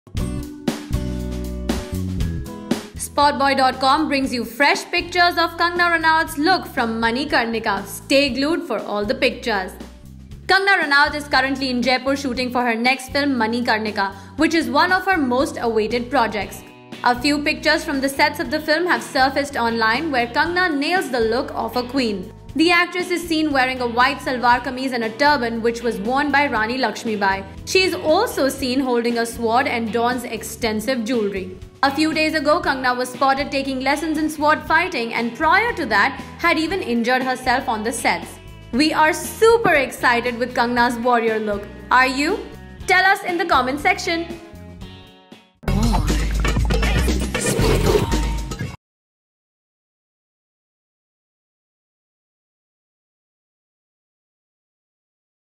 LEAKED! Kangana Ranaut’s Rani Laxmi Bai look from the sets of Manikarnika | SpotboyE
A few pictures of Kangana Ranaut’s ambitious project Manikarnika have found its way on the Internet, making us curious about what’s in store.